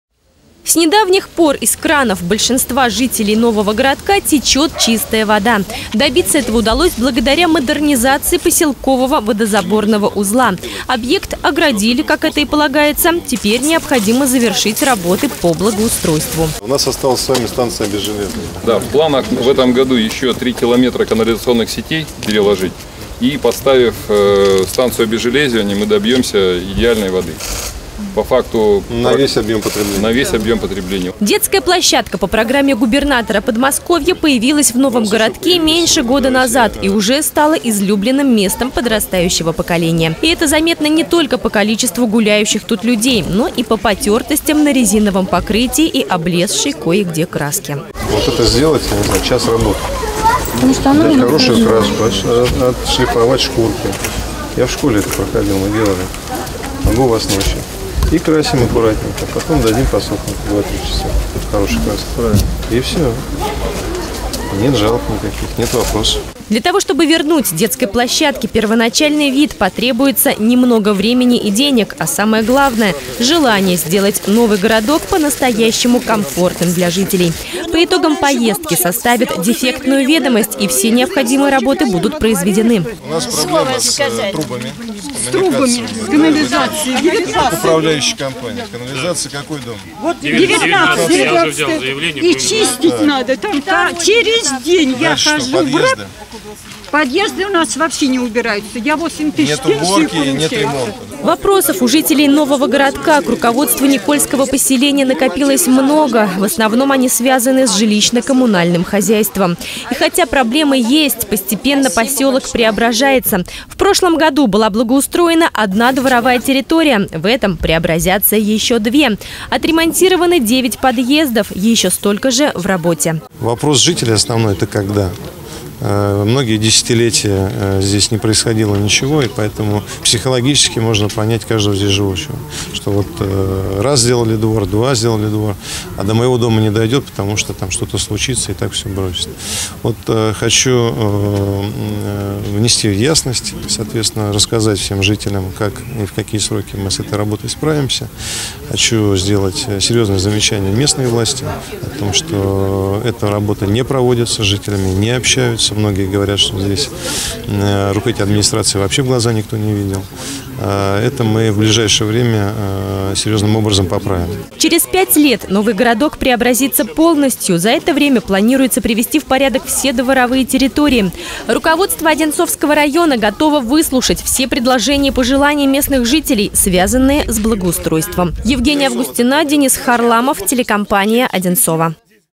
Глава района проинспектировал Новый городок
Водозаборный узел, детская площадка, жилые дома – глава Одинцовского района проинспектировал Новый городок. В ходе рабочей поездки Андрей Иванов проверил, как идёт благоустройство посёлка, а также пообщался с местными жителями, у которых было немало вопросов к руководству и района, и Никольского поселения.